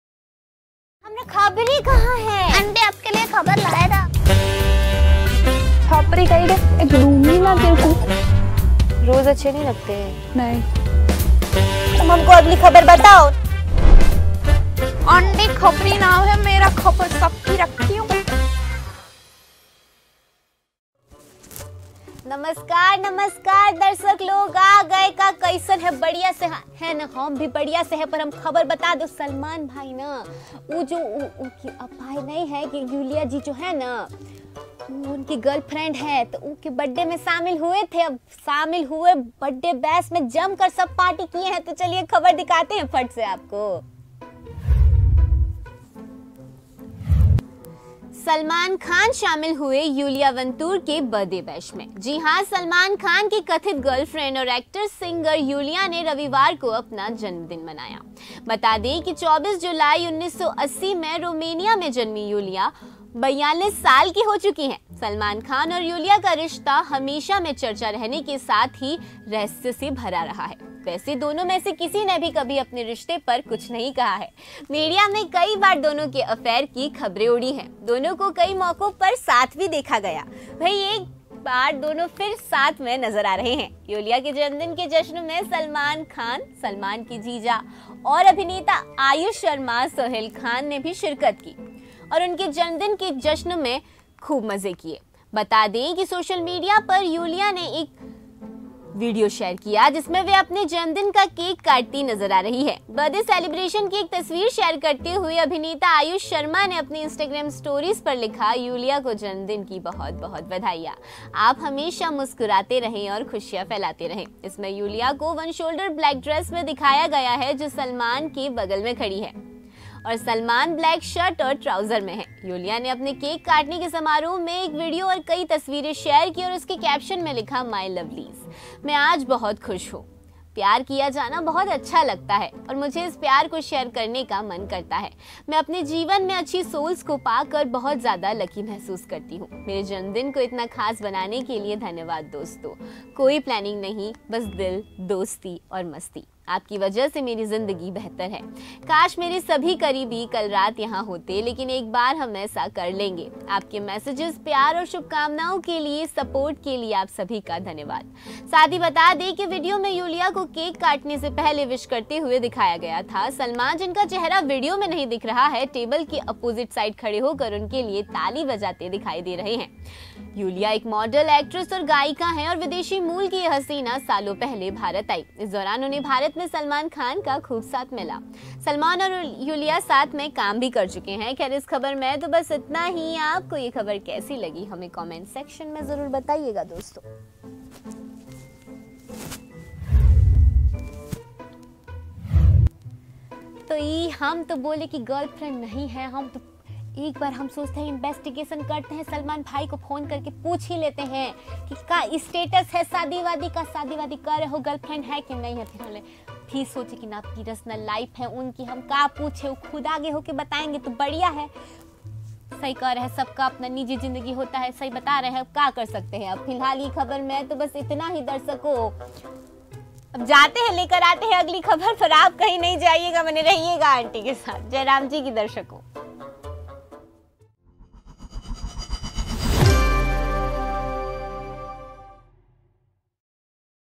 Salman Khan attends Yulia Vantur's birthday bash |सलमान खान शामिल हुए यूलिया वंतूर के बर्थडे बैश में
सलमान खान शामिल हुए यूलिया वंतूर के बर्थडे बैश में, जी हाँ सलमान खान की कथित गर्लफ्रेंड और ऐक्टर सिंगर यूलिया वंतूर ने रविवार को अपना जन्मदिन मनाया। बता दें कि 24 जुलाई 1980 में रोमानिया में जन्मीं यूलिया 42 साल की हो गई हैं.सलमान खान और यूलिया वंतूर का रिश्ता हमेशा चर्चा में रहने के साथ ही रहस्यमयी भी रहा है.